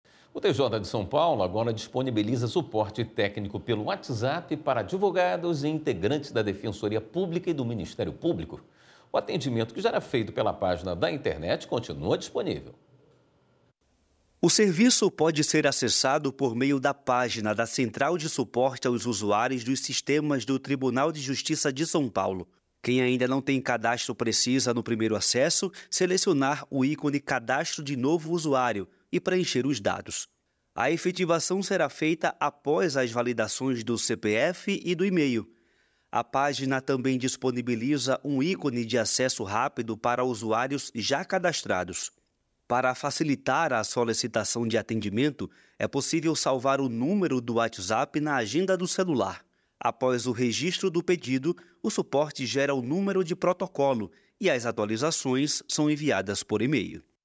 📺 JJ – TJSP disponibiliza suporte técnico pelo WhatsApp para advogados
O novo canal de comunicação também poderá ser utilizado para suporte de integrantes da Defensoria Pública e do Ministério Público. O atendimento pela internet continua disponível.